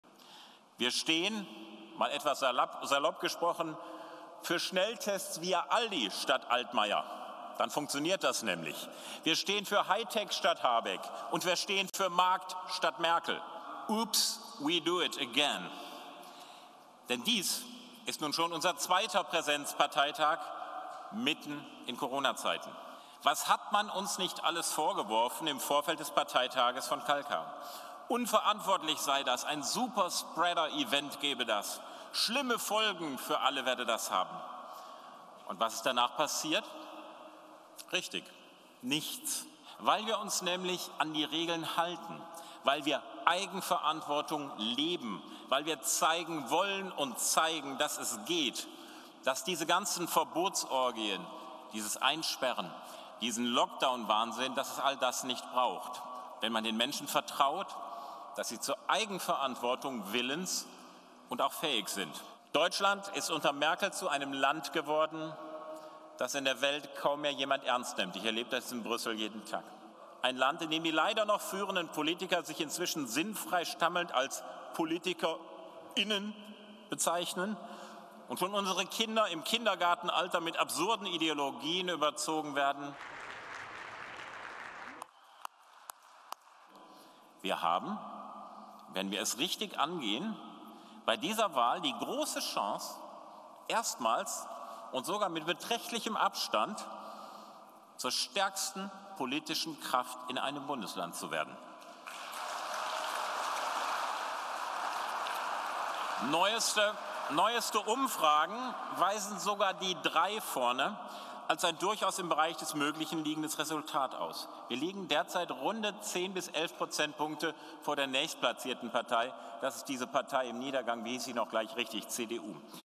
„Ups, we do it again“: Meuthen zum Auftakt des AfD-Parteitags
Die AfD hielt am Samstag ihren Bundesparteitag in Dresden ab. In diesem Jahr stehen viele Wahlen an. Bundessprecher Jörg Meuthen machte den Auftakt.